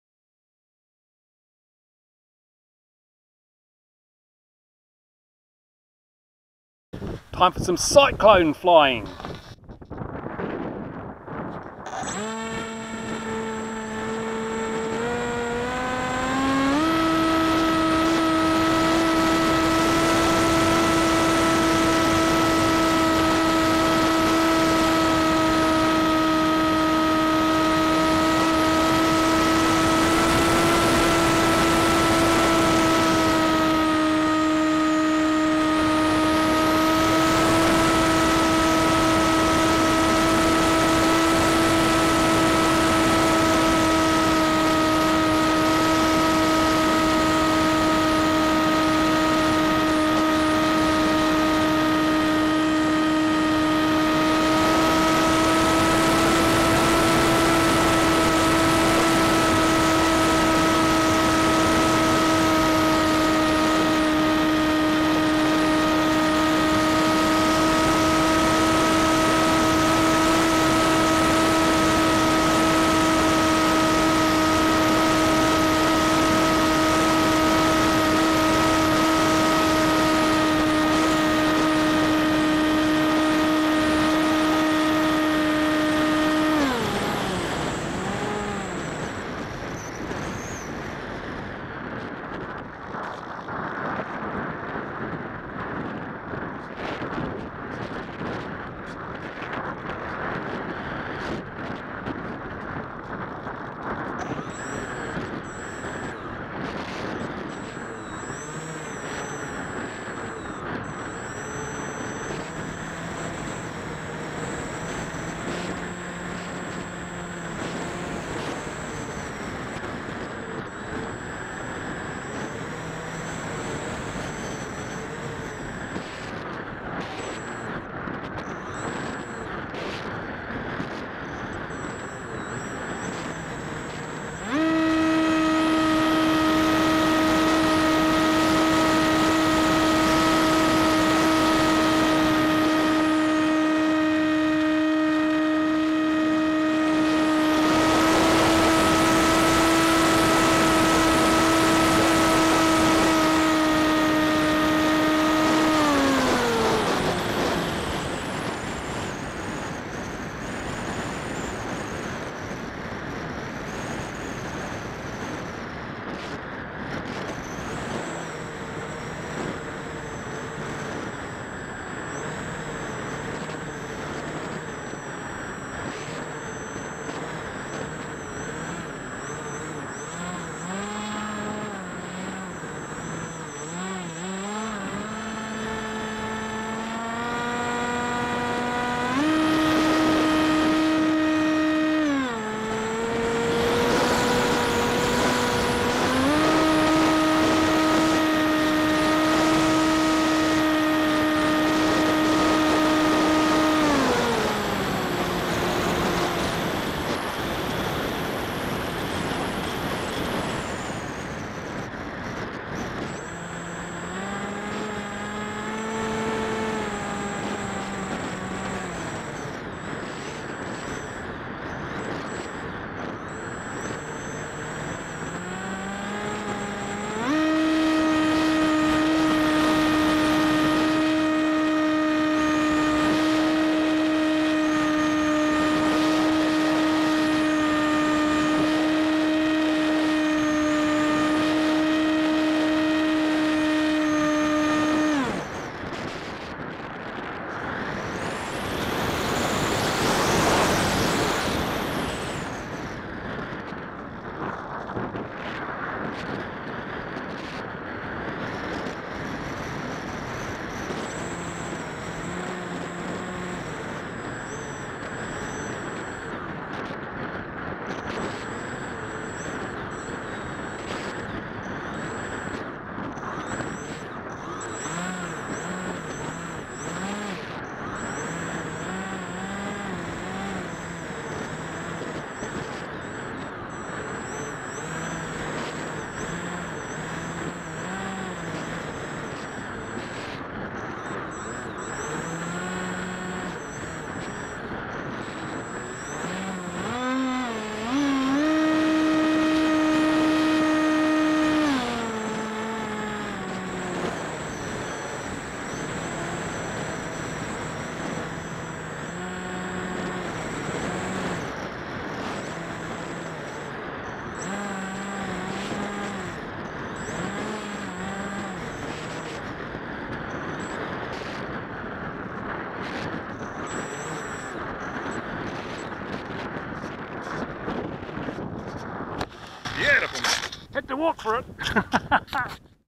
RC plane + tropical storm + onboard mobius camera
Fly an AXN RC plane in a tropical storm (was tropical cyclone Lusi?).  Of course!

Bah... but what a fizzer!

The winds were far lower than predicted but it sure *was* turbulent so I had a go anyway.   Not much to see here really except you can marvel at the great performance of the Mobius 1080p video camera.